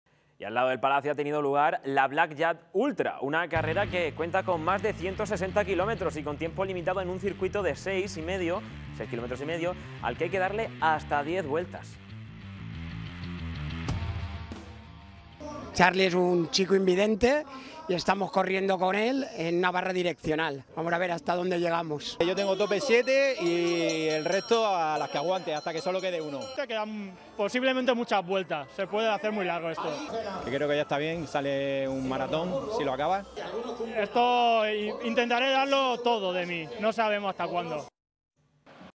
La carrera más impresionante del mundo llega a Cartagena | La 7
La Blackyard Ultra aterriza en la ciudad portuaria por primera vez, una carrera americana con más de 160 kilómetros con la peculiaridad que se hace en un circuito cerrado. La7 se ha puesto las zapatillas y ha echado a correr con las historias que nos ha dejado el Trail.